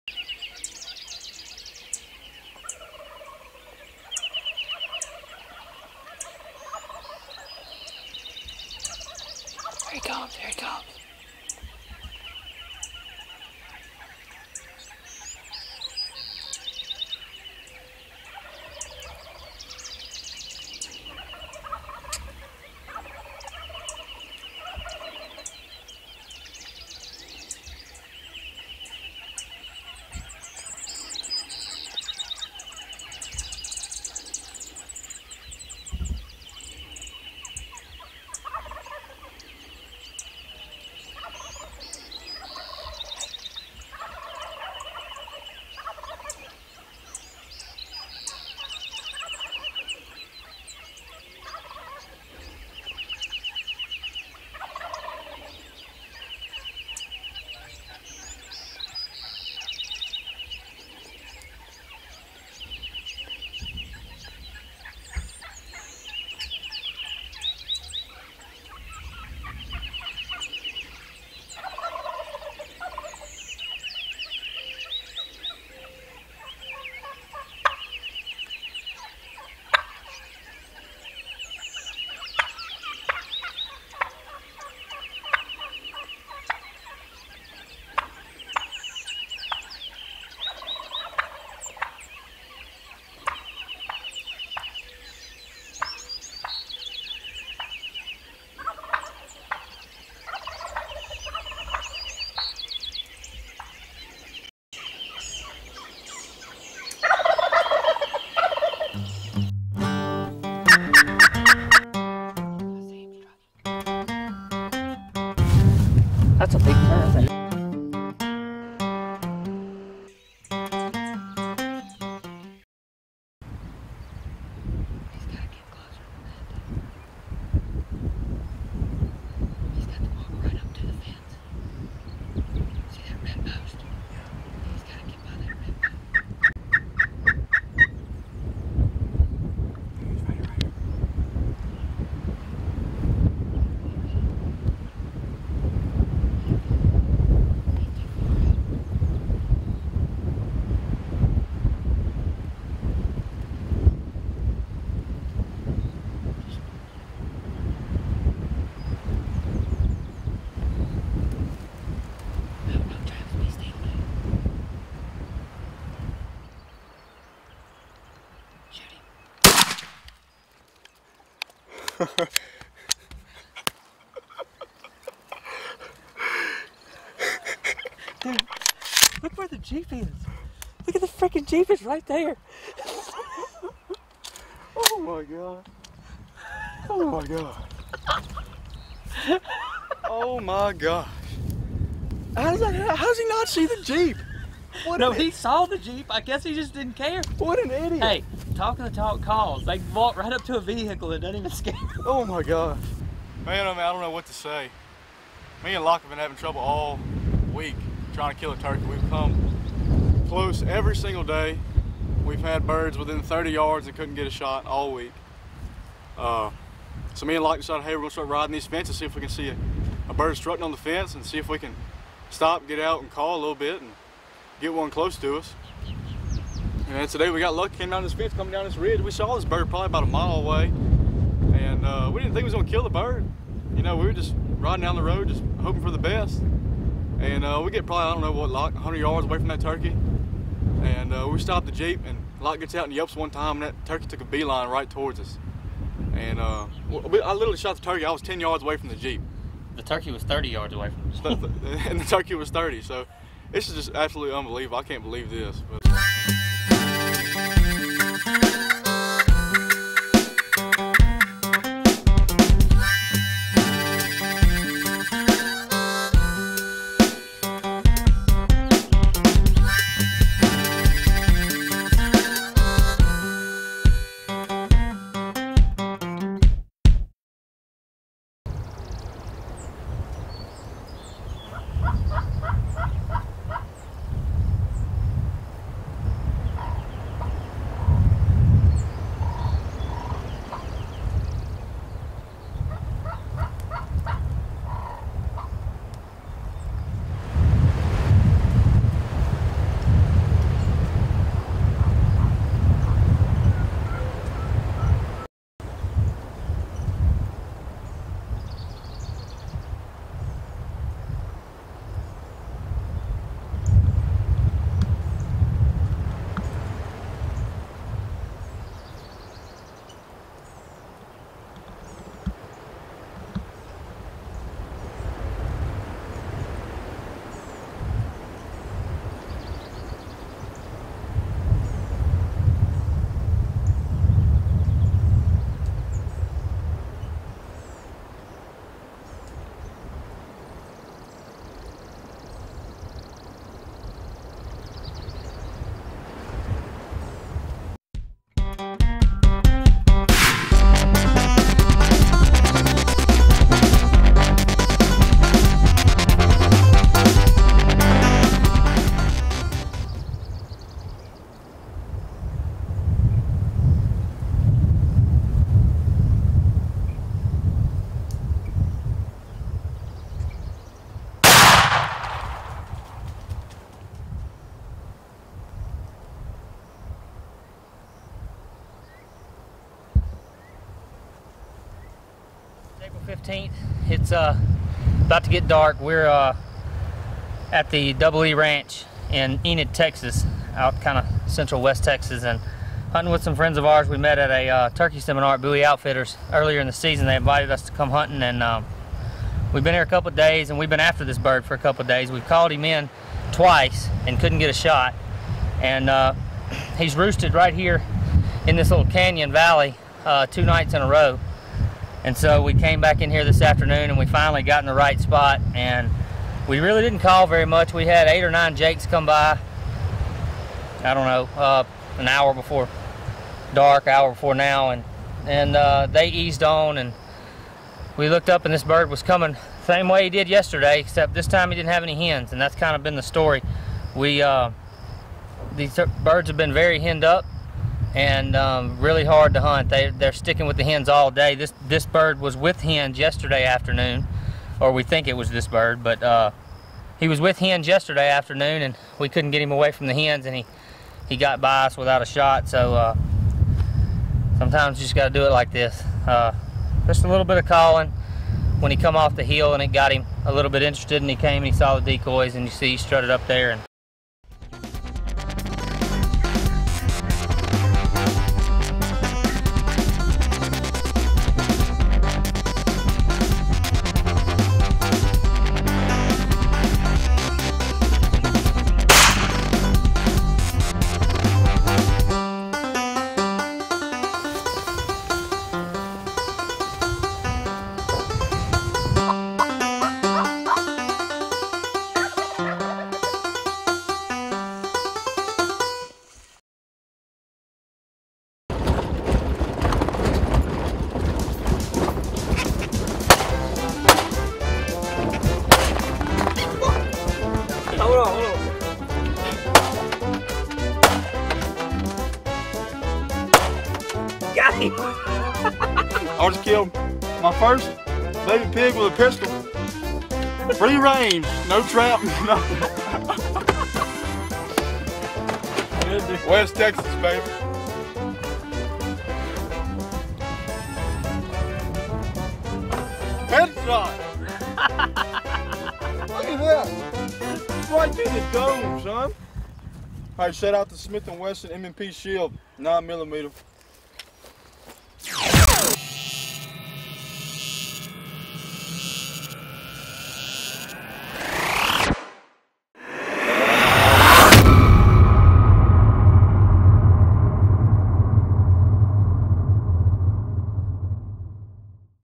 The Double EE
T3 crew invades west Texas to chase Rio Grande gobblers